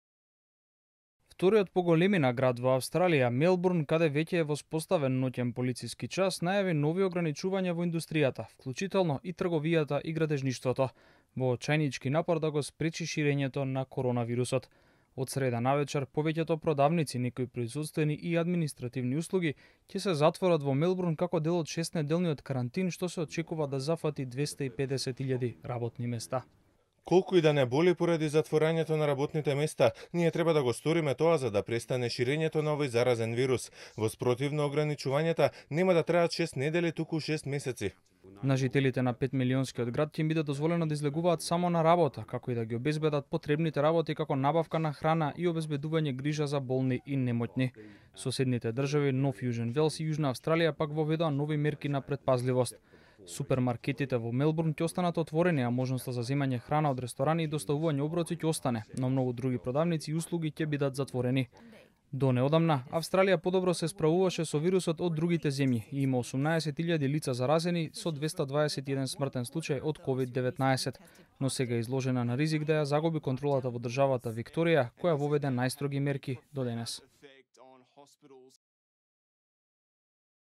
Мелбурн под целосен карантин, не работат ниту продавниците
Австралија воведува драконски мерки во обид да го спречи ширењето на коронавирусот...Во вториот по големина град во Австралија, Мелбурн, најавени се нови ограничувања и полициски час.